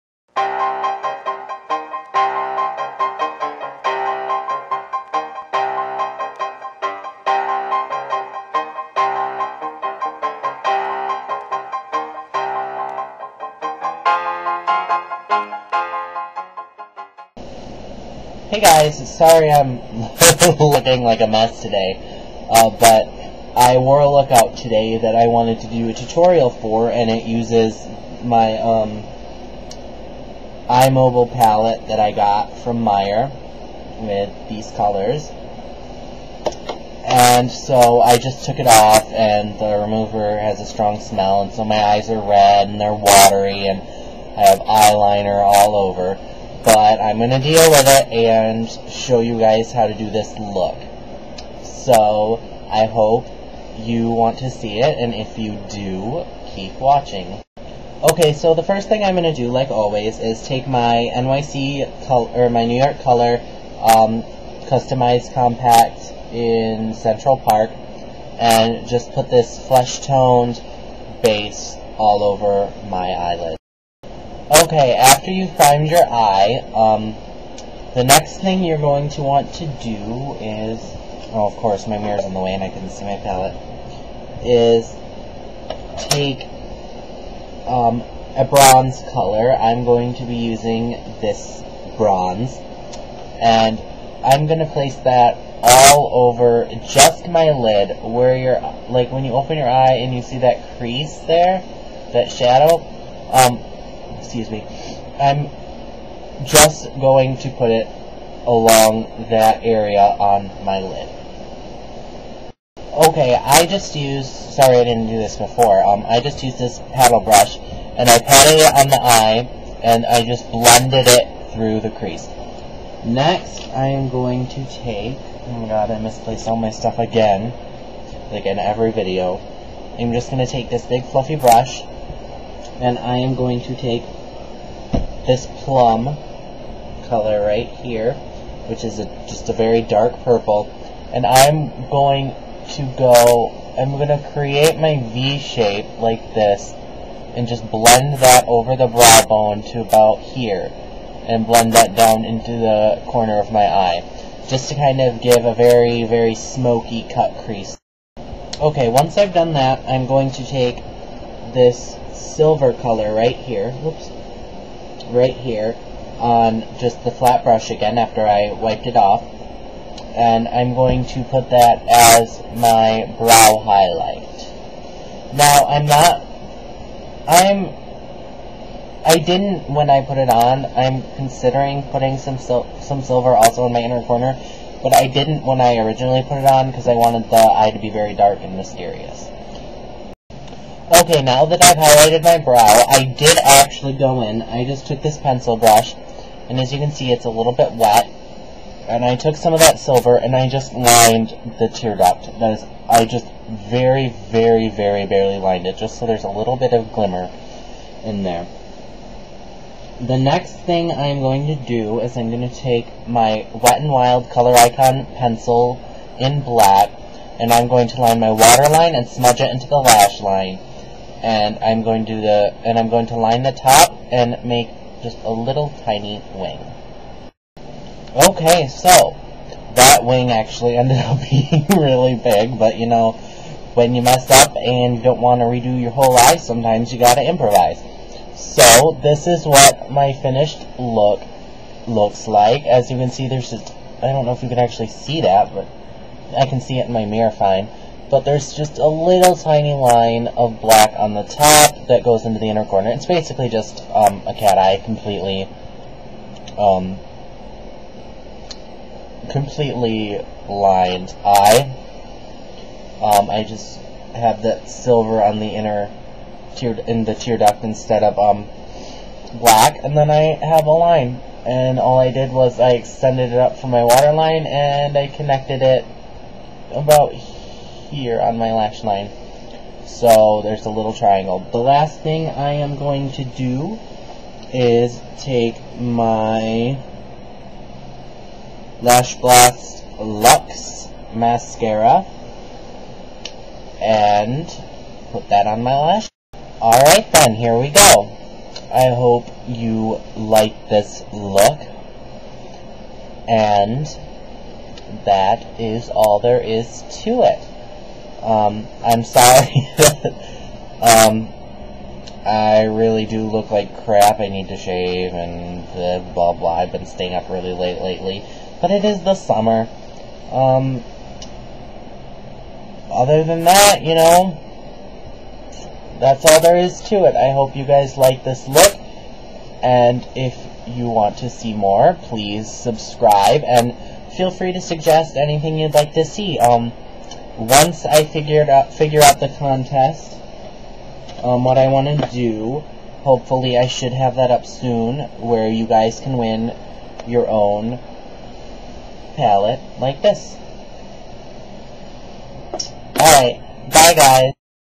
LOOK: Dark Lady - Bronze, Purple, and Silver.
Hi guys!  Here's a bronze, purple, and silver smokey eye with a cut crease.  I hope you guys like it!  What do you think of the intro, by the way?

Products used:
N.Y.C. Custom Eyes Compact in 940U Central Park
The Color Workshop Eye Mobile 20 Color Pallet
Wet'N'Wild Coloricon Pencil in Black